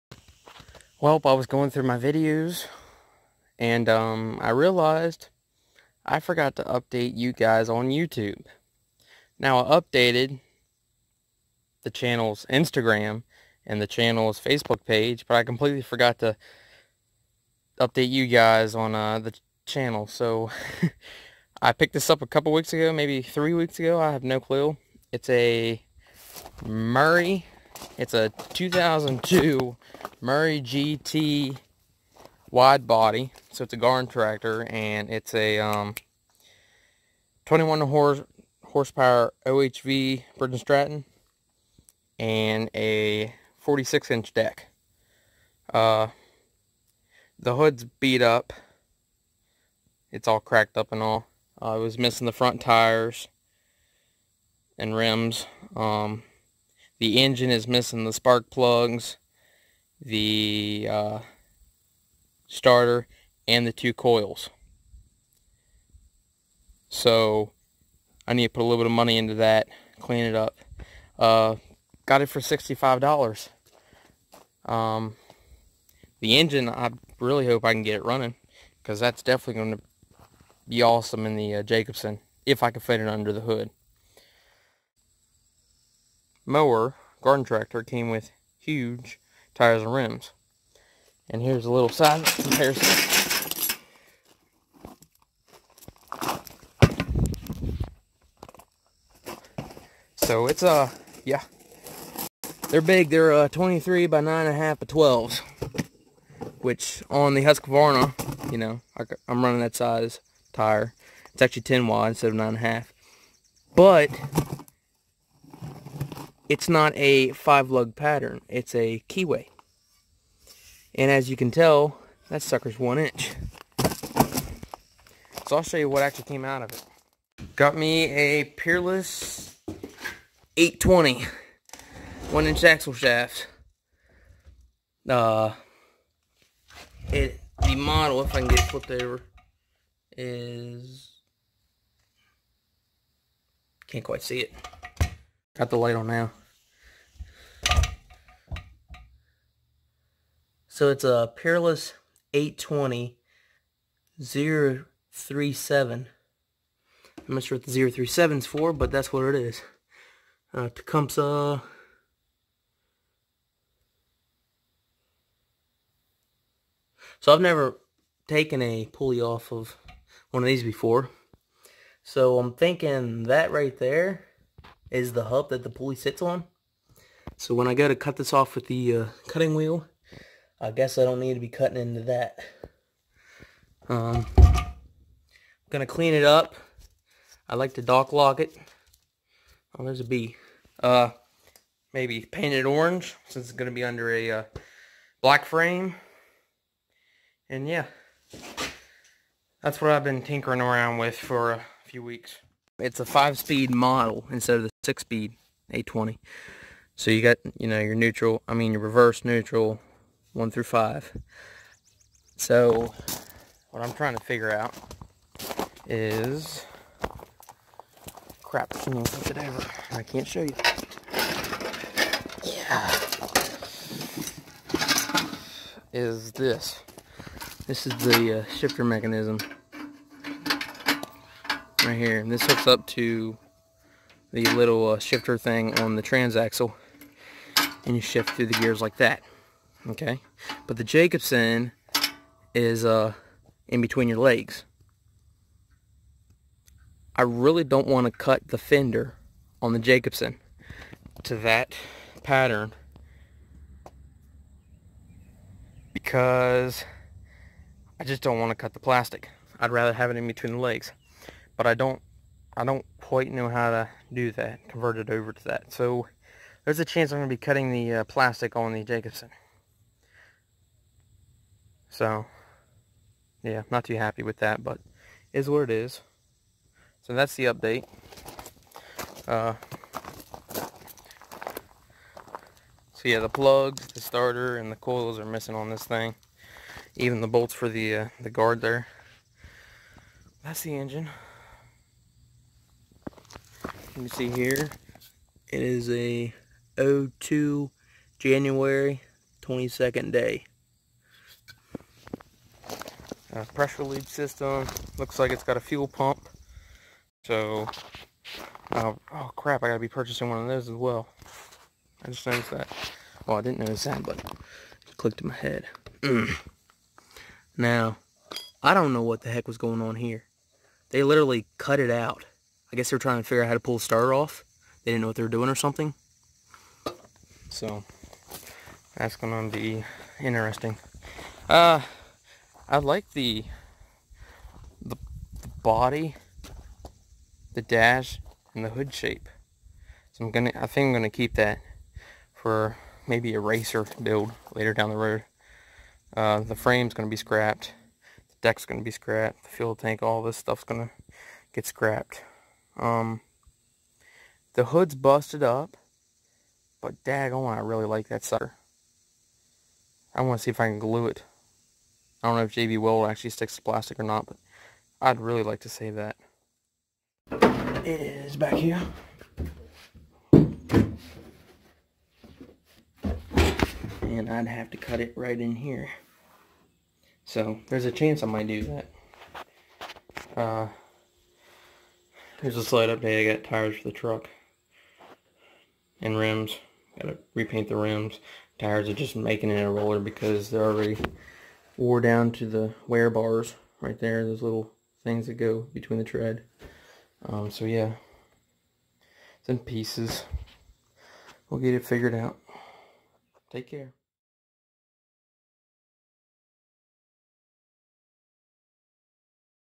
2002 Murray Widebody GT 21HP/46" & A Peerless 820
Got my hands on some great parts for $65.00. The Peerless 820 and the 21hp Briggs and Stratton V-TWIN is going in the ATLTF 1988 Jacobsen Homelite LT10 Build.